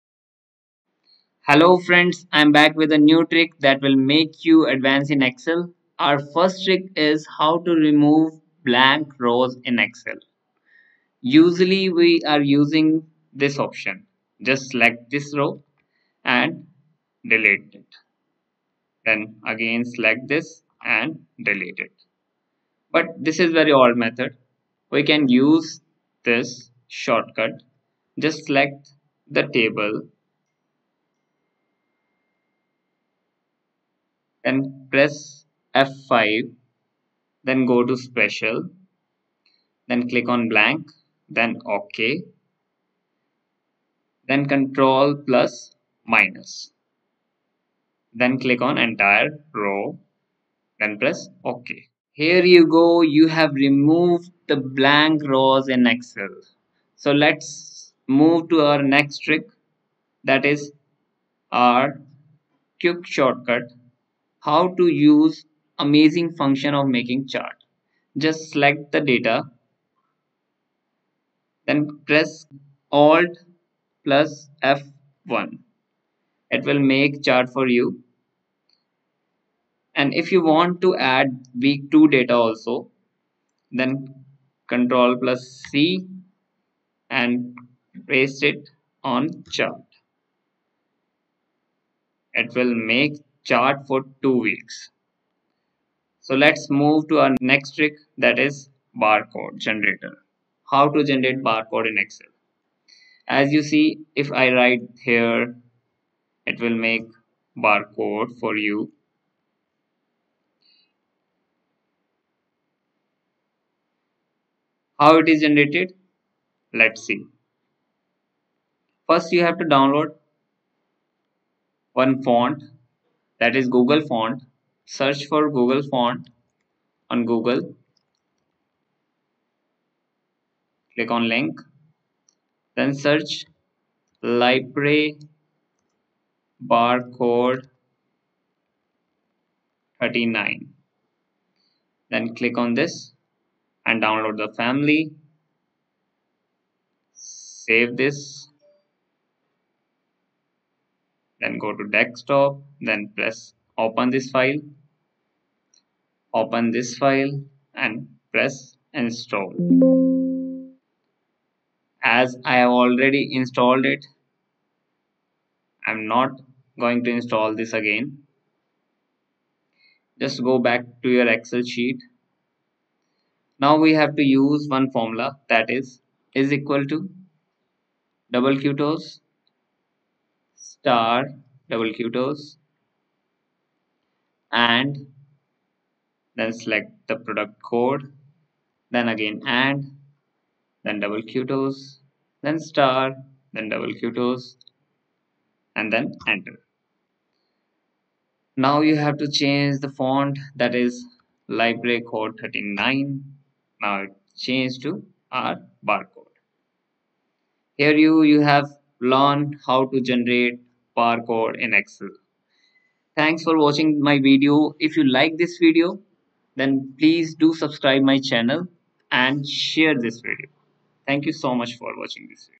3 Excel Tricks and Tips | Learn Ms-Excel
In this video, you will learn 3 excel tricks and tips that will make you advanced in excel.